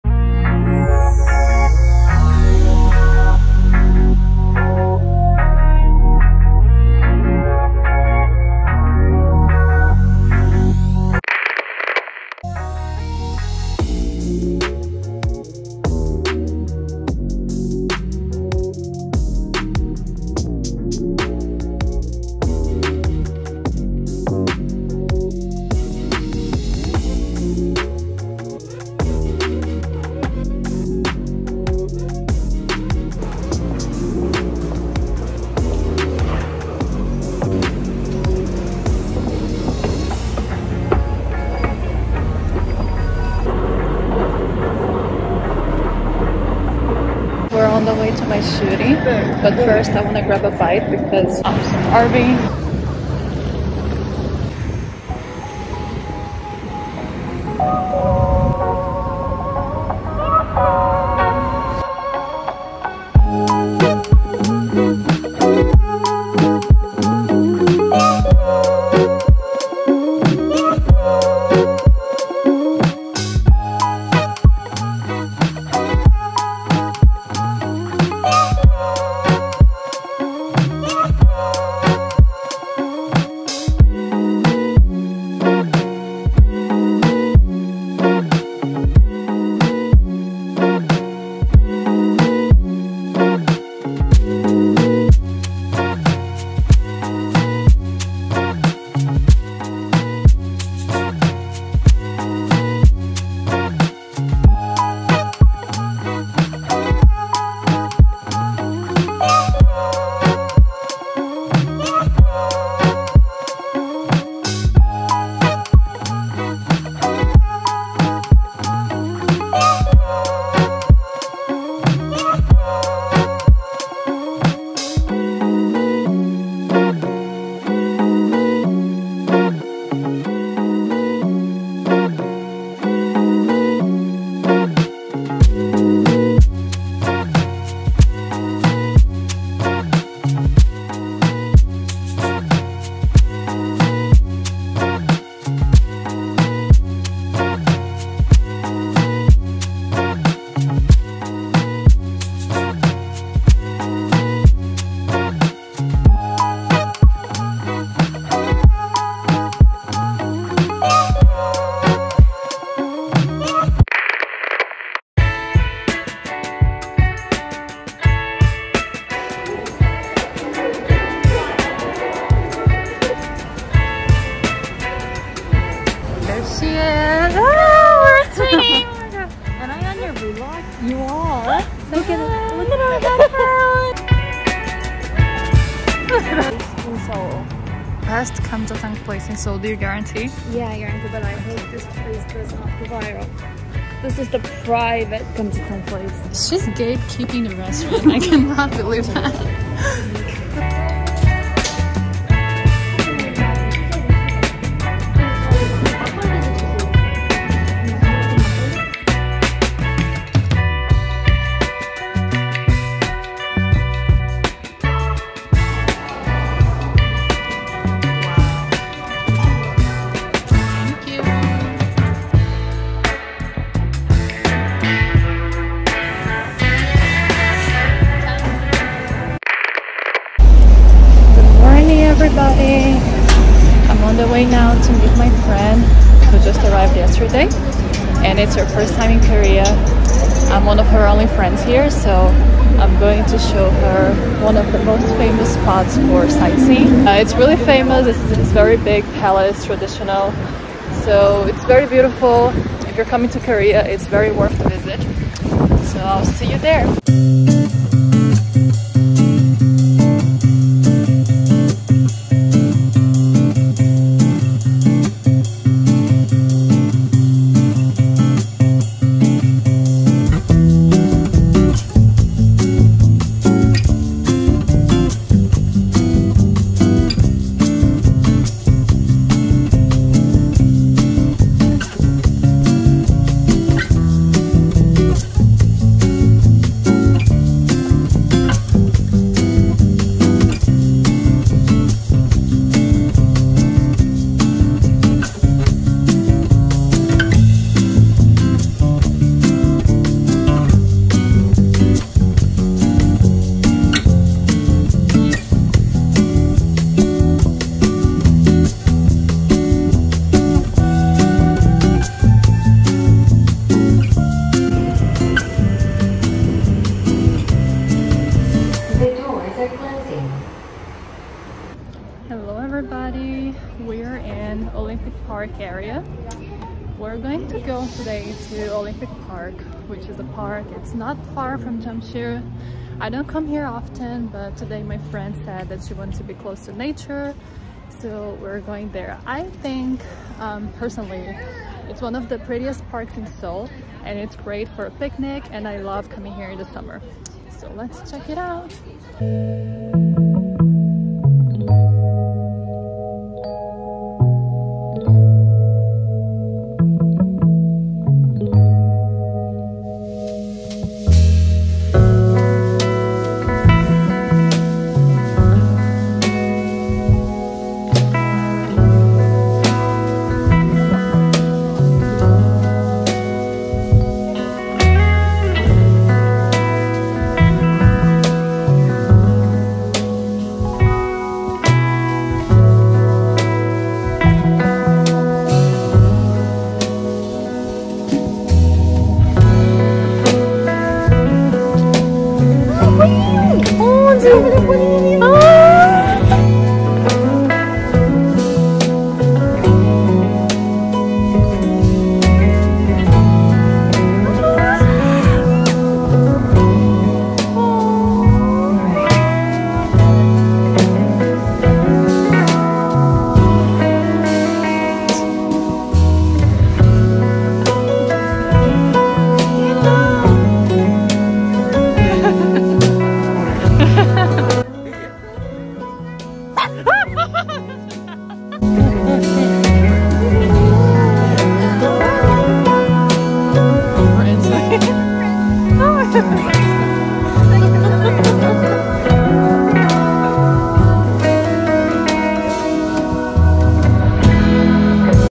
vlog 🇰🇷 | modeling in Korea, traditional palace, brunch, olympic park stroll
a little backstage from one of my shootings, trying traditional Korean food, taking my friend sightseeing and more!